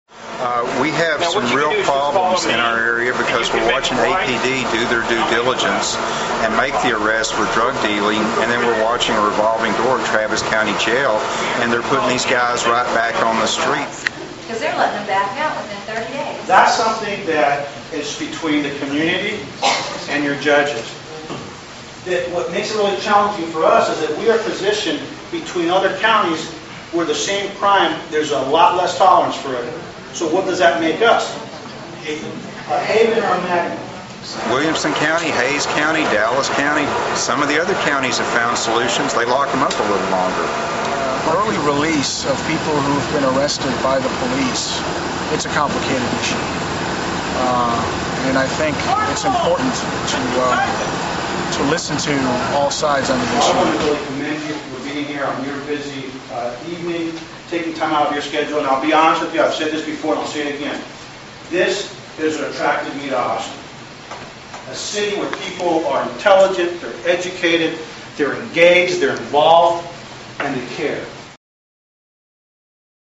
Repeat offenders back in neighborhood
Residents in the part of town south of the Colorado River and east of Interstate 35 shared their concerns of repeat offenders in their neighborhoods with the police department at a neighborhood forum.  Miguel Liscano AMERICAN-STATESMAN